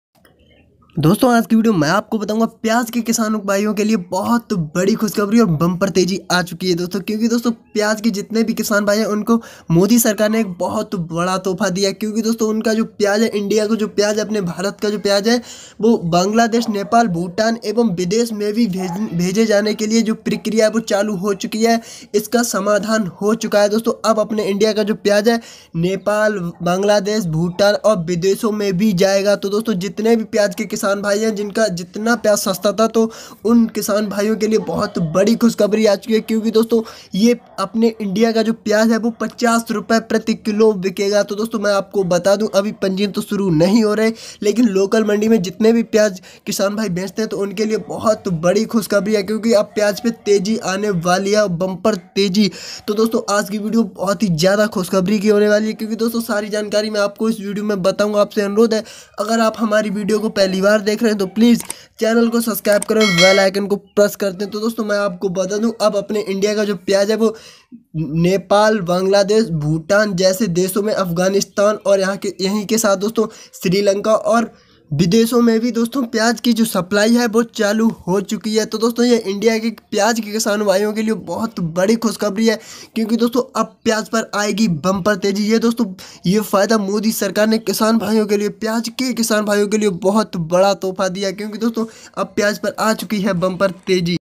प्याज मंडी ताजा भाव| प्याज किसानों को कृषि मंत्री का तोहफा खुशखबरी, payaj bhav, onoin rate today
मध्य प्रदेश की जो सरकार है उसने लाभ अंतर योजना के तहत प्याज खरीदेगी बहुत बढ़िया बेटा प्लीज सबसे वीडियो को पूरा देखें तभी समझ पाएंगे पूरी कहानी